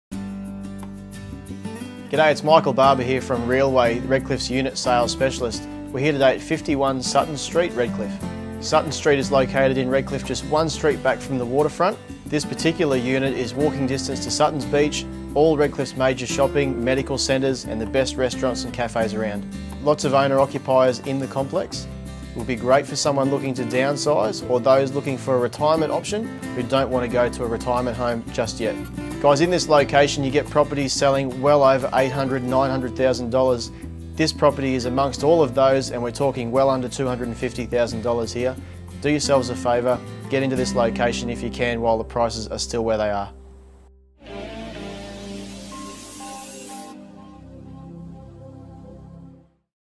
Redcliffe Real Estate - 3/51 Sutton Street, Redcliffe - RealWay Peninsula Units -- Michael Barber
RealWay Property Consultants Redcliffe

Ground Floor Unit In The Heart Of Redcliffe

Amidst the beautiful gardens of this well maintained complex of units is this 2 bedroom unit, ready to be yours.

With the convenience of shopping, medical centres, dining and specialty stores all at your doorstep it is a perfect place for those looking to retire, without the huge fees that usually come with it.

The property features built in robes in both bedrooms, ceiling fans, security screens and a good sized covered patio area with privacy fencing all around.

The property is in good condition, but there is also room for some light cosmetic renovation for the smart investor to really capitalise.

These units in this proximity to the water never last long so act fast to secure it today.